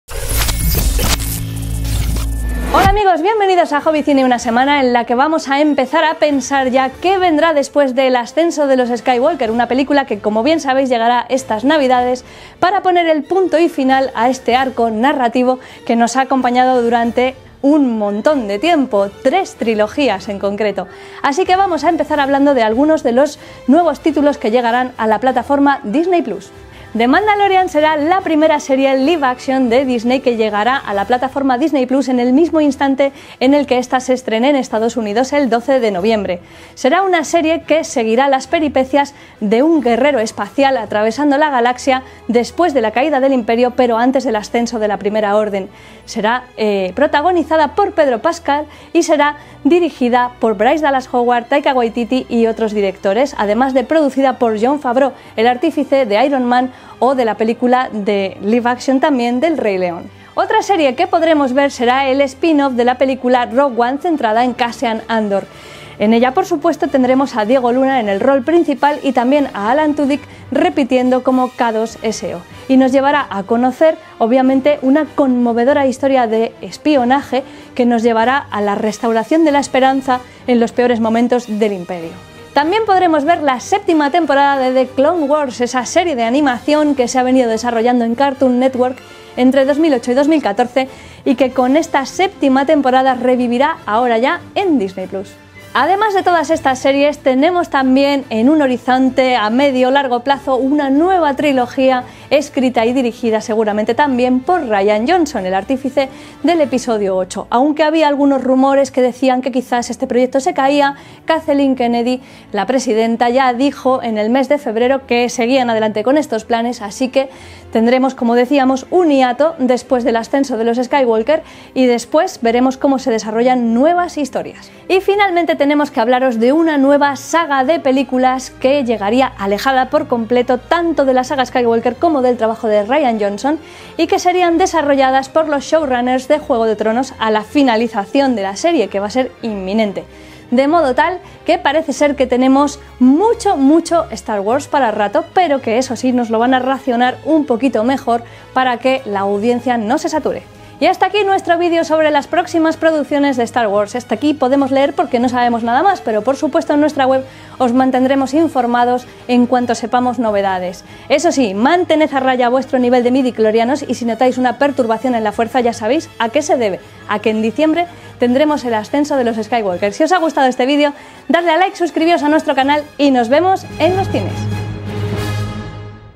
Star Wars - Todas las películas y series más allá de la saga Skywalker
Esta semana repasamos los planes de Disney para la saga Star Wars: todo lo que sabemos de las próximas películas y series que llegarán a cines y a Disney+.